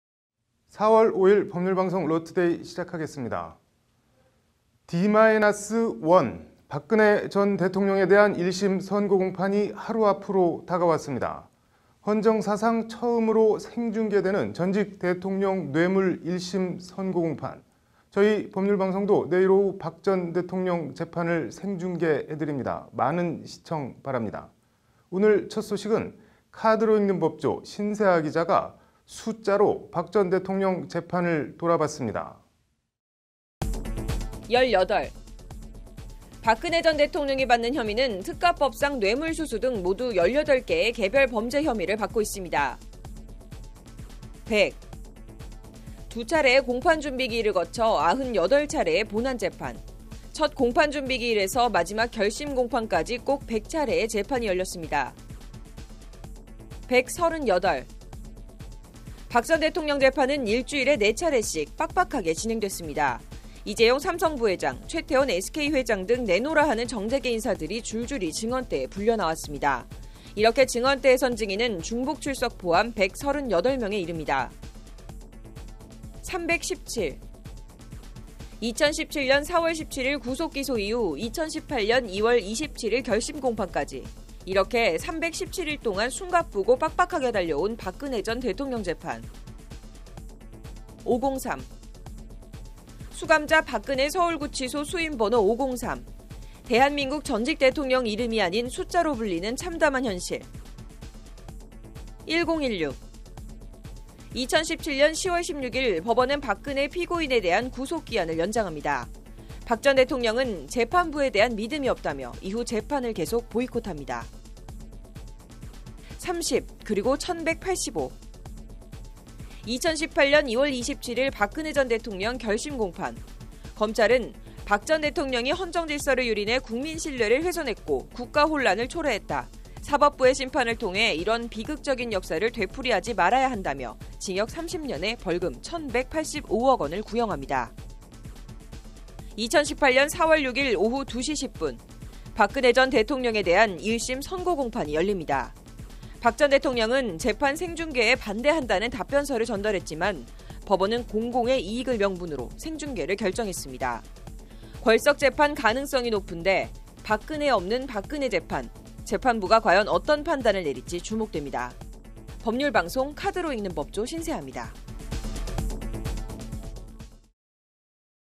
박근혜 1심 선고 D-1... '숫자로 본' 317일 간의 朴 재판 과정
D-1, 박근혜 전 대통령에 대한 1심 선고공판이 하루 앞으로 다가왔습니다. 숫자로 박 전 대통령 재판을 돌아 봤습니다.